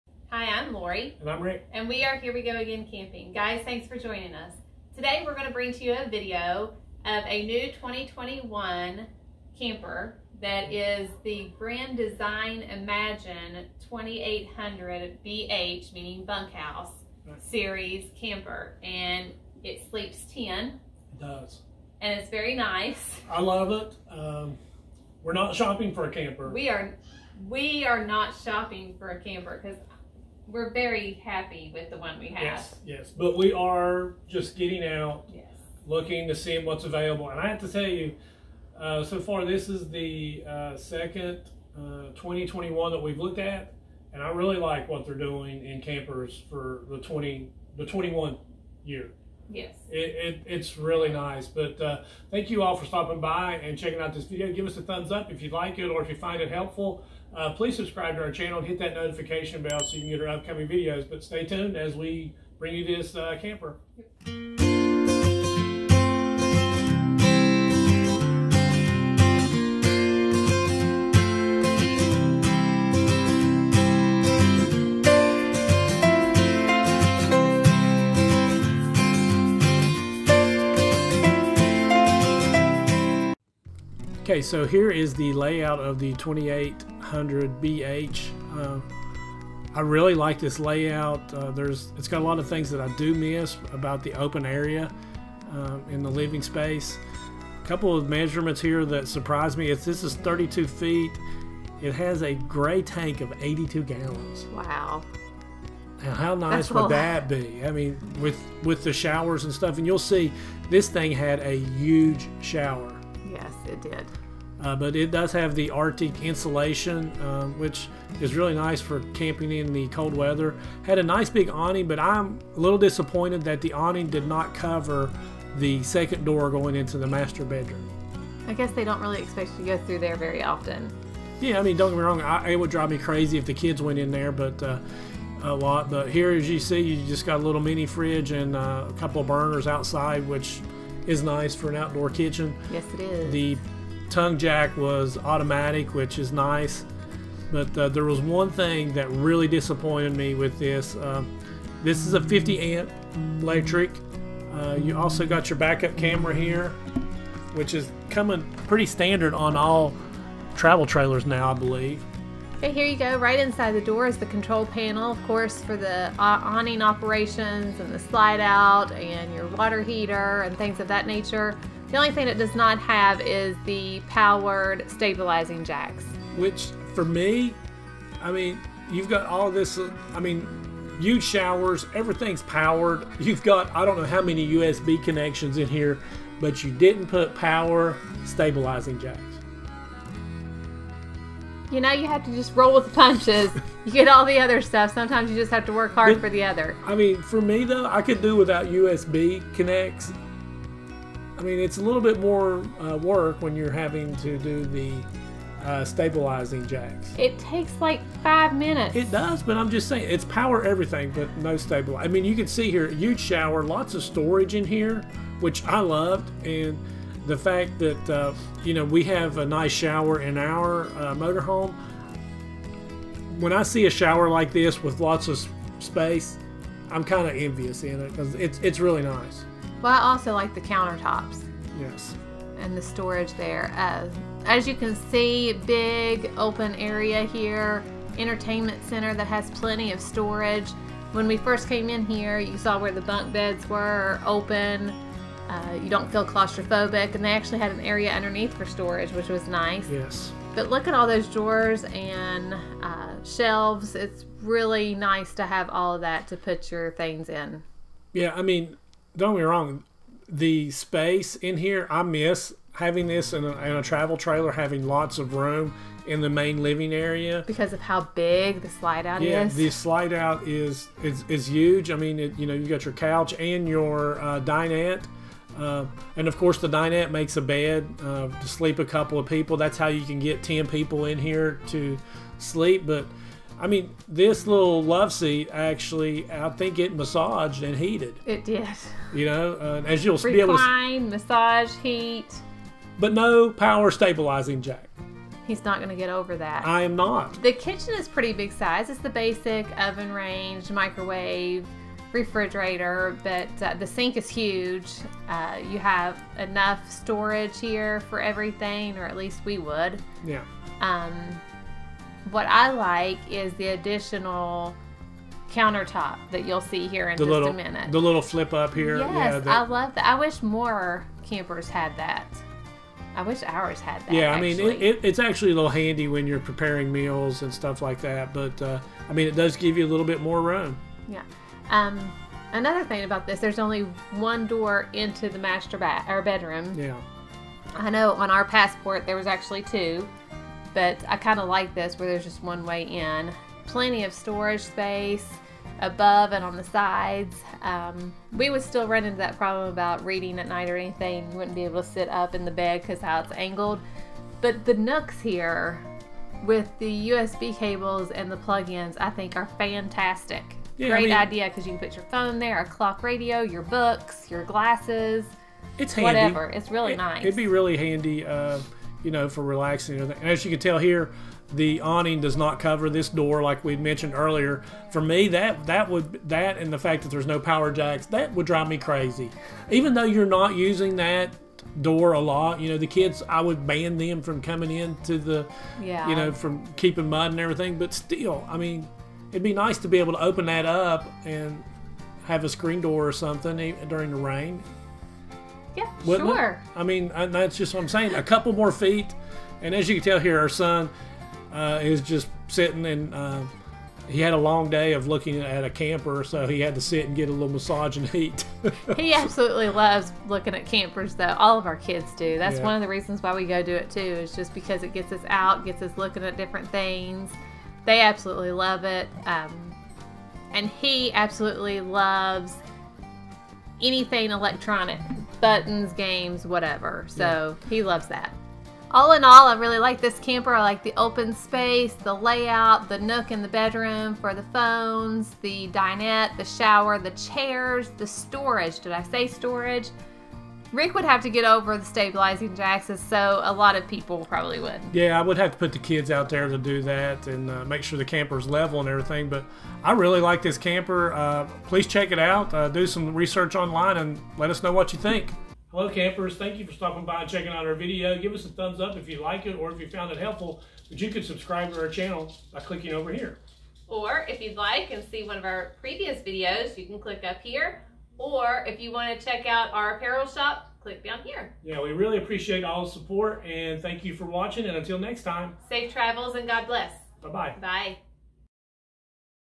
Camper tour 2021 Grand Design Imagine 2800BH
Today we are walking through a 2021 Grand Design Imagine 2800BH.  Love this camper for the layout and storage.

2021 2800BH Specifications
Sleeps 10
Slides 1
Tire Size 15"
Furnace BTU 35000 btu
Number Of Bunks 2
Available Beds Queen
Refrigerator Type Double Door
Cooktop Burners 3
Shower Size 30" x 36"
Number of Awnings 1
Axle Weight 4400 lbs
LP Tank Capacity 20 lbs
Water Heater Capacity 6 gal
Water Heater Type Quick Recovery
AC BTU 15000 btu
TV Info LR LED HDTV
Awning Info 19' Power Adjustable w/LED Lights